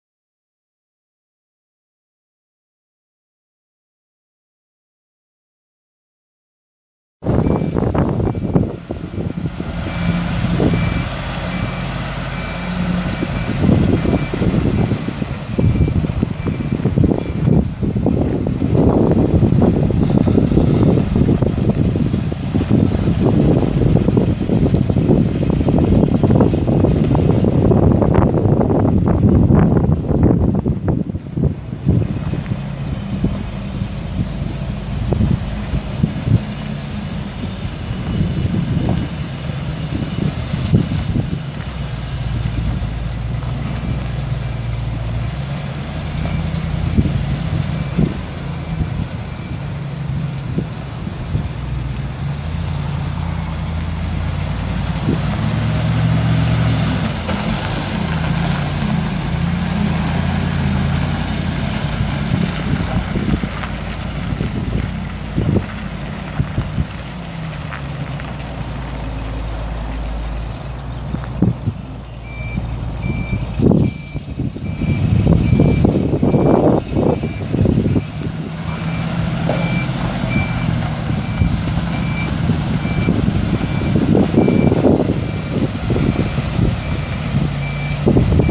1989 Chevrolet Custom Deluxe C7D042 bucket truck for sale | no-reserve auction April 28, 2016
View a detailed listing of this item and place bids

1989 Chevrolet Custom Deluxe C7D042 bucket truck
26,229 miles on odometer
1,352 hours on meter
Approx. 4,689 total hours
6.0L V8 gas engine
Beacon light
Work lights
1994 Telelect T50501 boom
50' boom length
Double man basket
(4) outriggers
Platform boom controls
Boom rotation
Material handler
Serial T5000-732BW
Pintle hitch
Spring suspension
Goodyear 10.00-20 tires
Non-operational boom
Hoses need to be replaced

South Dakota
1GBL7D1B8KV108859

1GBL7D1B8KV108859

Location
Muth Electric Inc.

Item location address: This item is located at 701E 48th Street North, Sioux Falls, SD